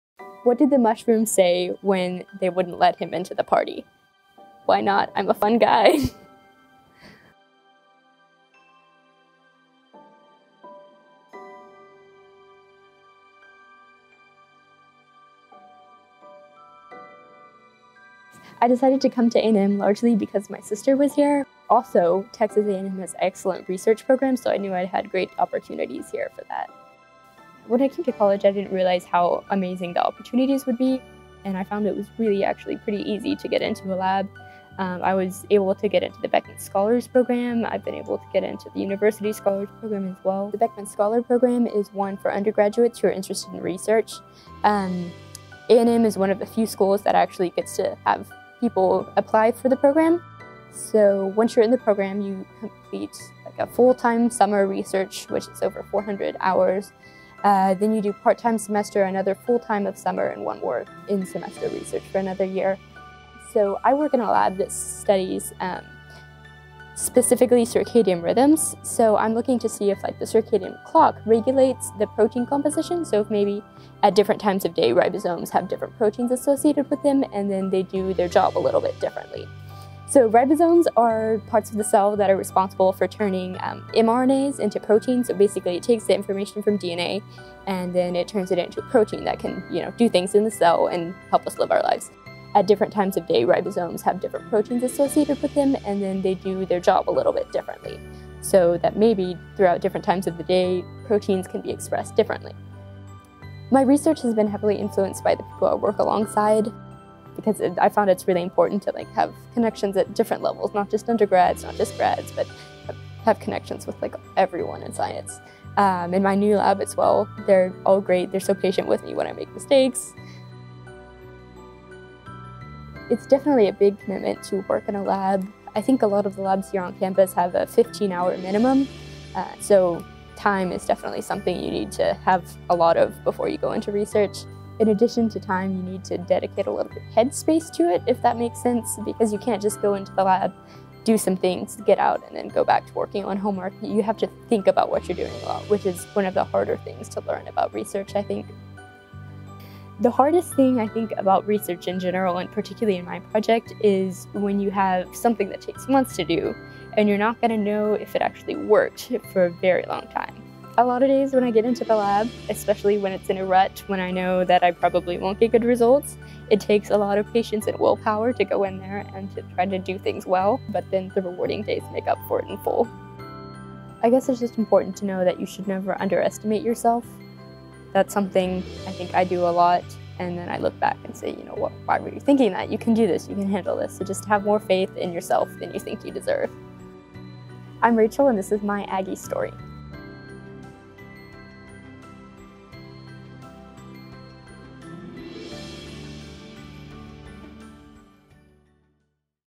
My Aggie Story: Rachel Porter on the Circadian Clock
Rachel Porter is currently a Beckman Scholar doing research on the Circadian Clock- looking at if the Circadian clock regulates protein compositions at different times of the day.